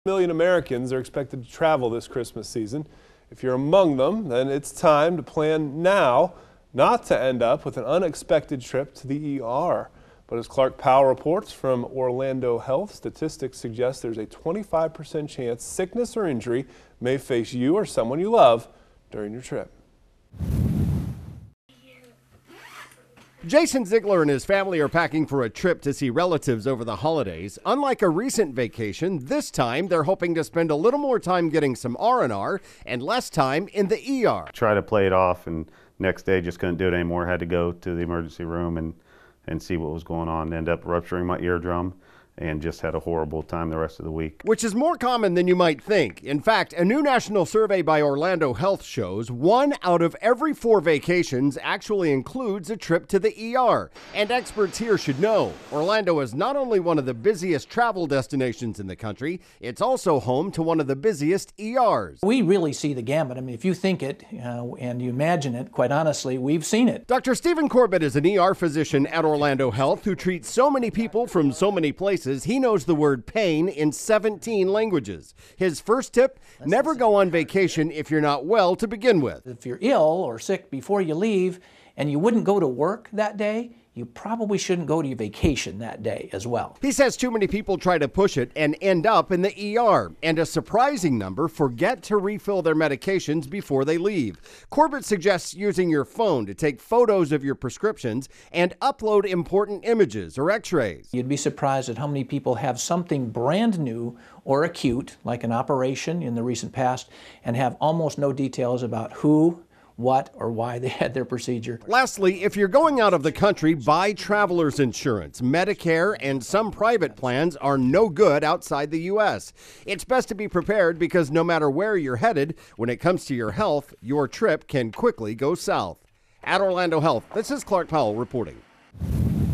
Avoiding the ER During Vacations
Clark Powell reports from Orlando Health